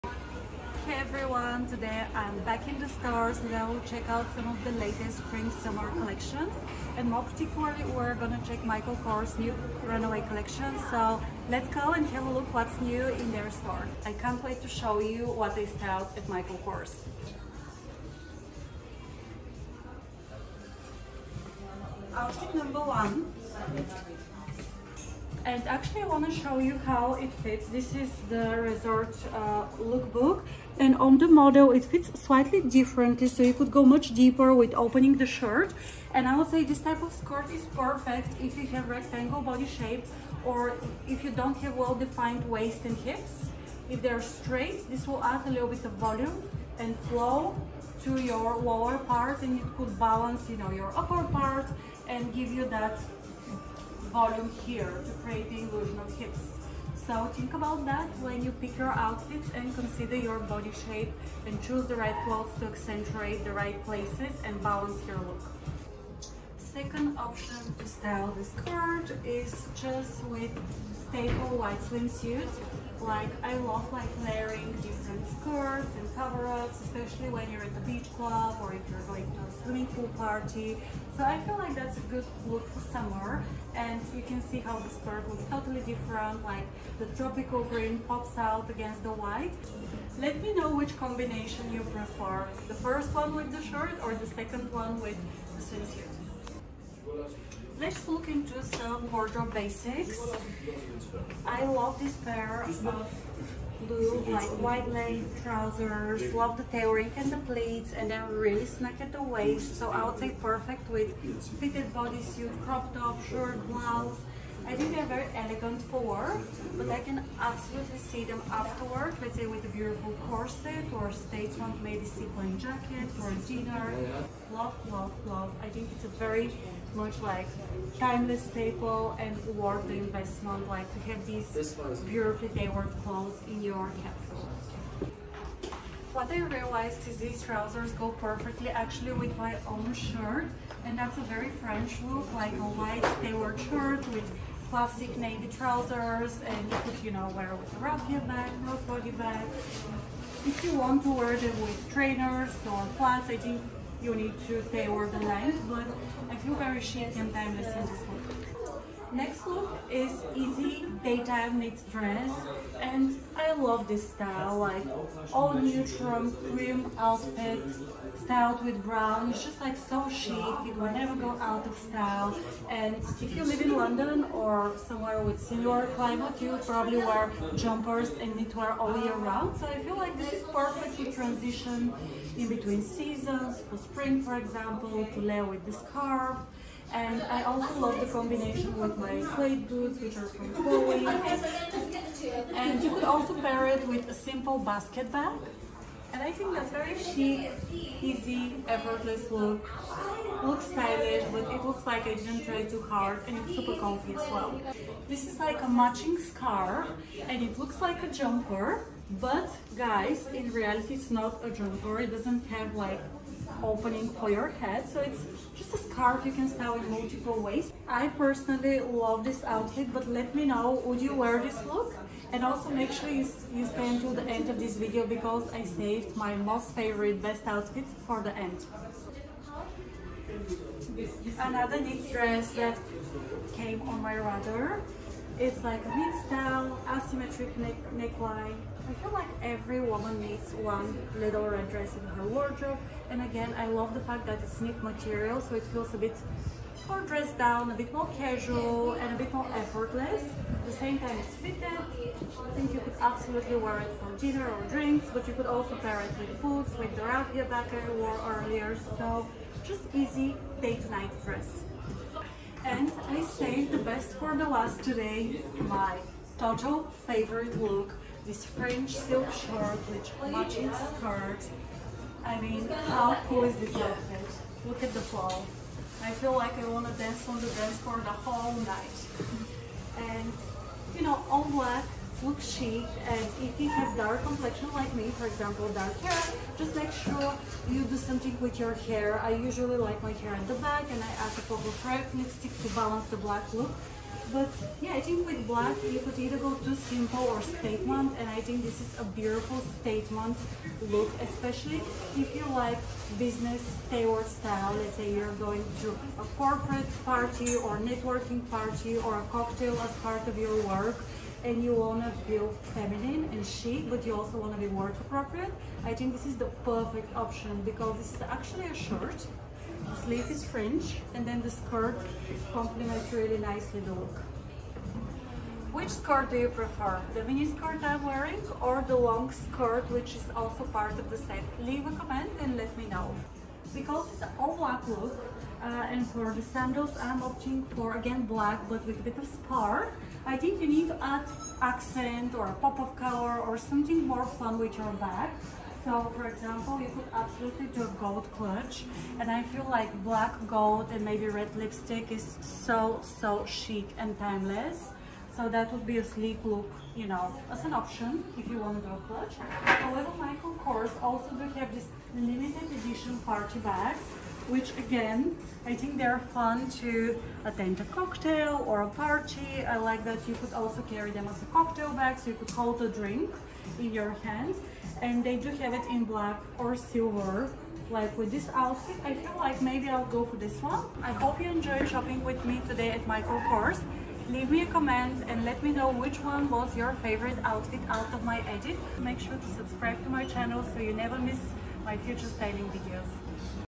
Visit at Michael Kors: 5 outfits you can NOT miss
Discover the Michael Kors Spring/Summer 2023 runway collection with the personal stylist Deni Kiro.